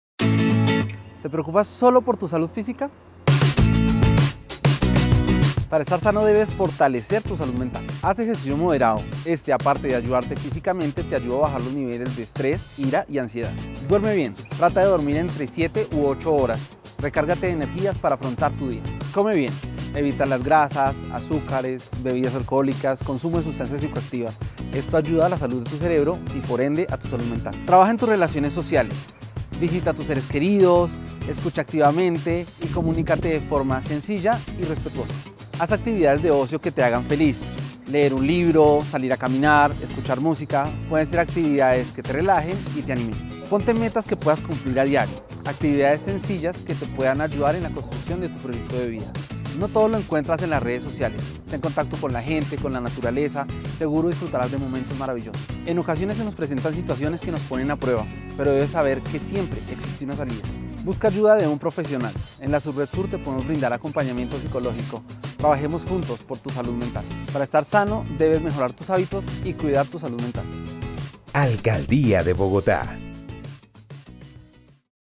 Para estar sano debes... Cuidar tu salud mental
Profesionales de la Subred Sur ten enseñan los cuidados que debes tener para cuidar tu salud mental